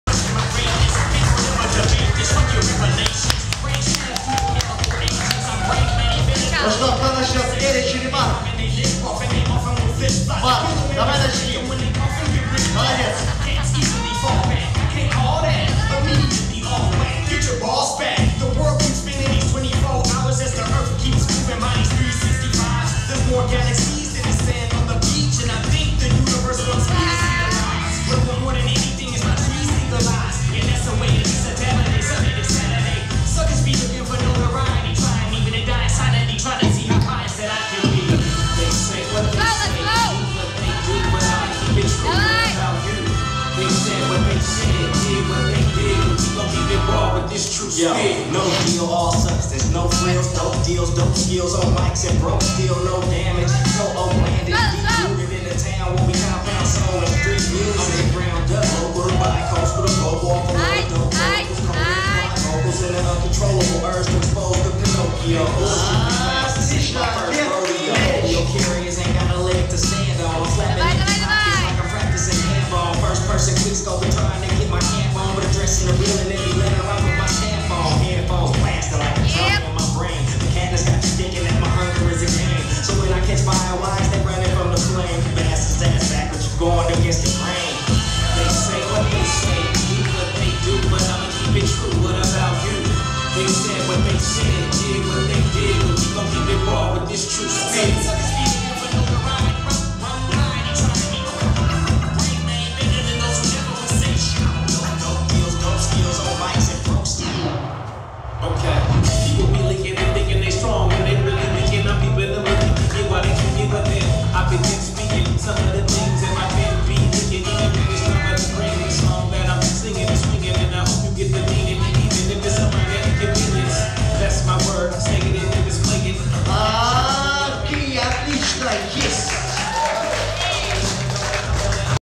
Лерич vs Марк | 1/4 | HIP-HOP advanced | Dance2Fly Contest 2
Dance 2 Fly Contest
Saint-Petersburg, Russia
25 December, 2016

Love your dance